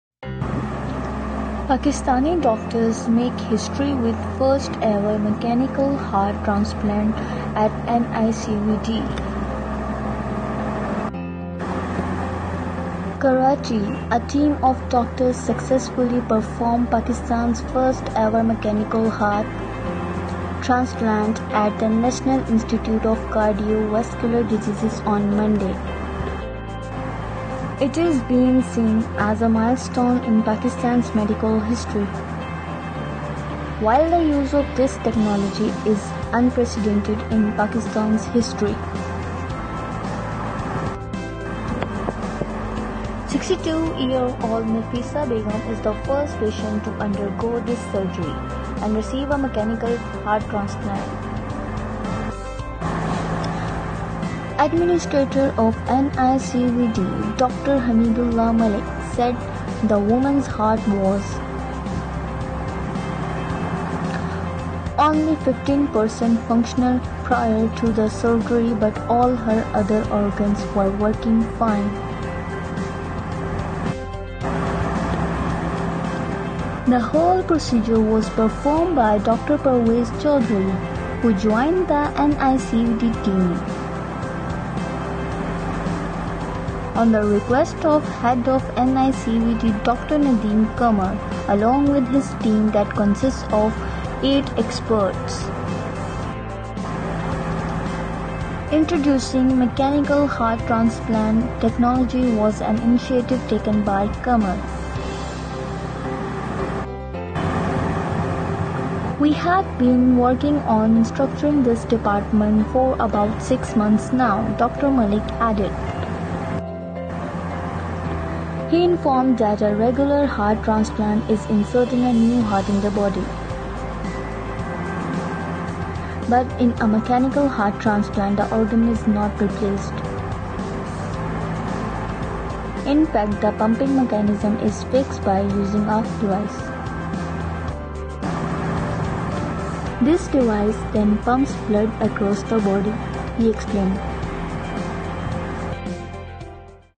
Pakistani Doctors make history with heart transplant
Pakistani Doctors make history with first ever mechanical heart transplant at NICVD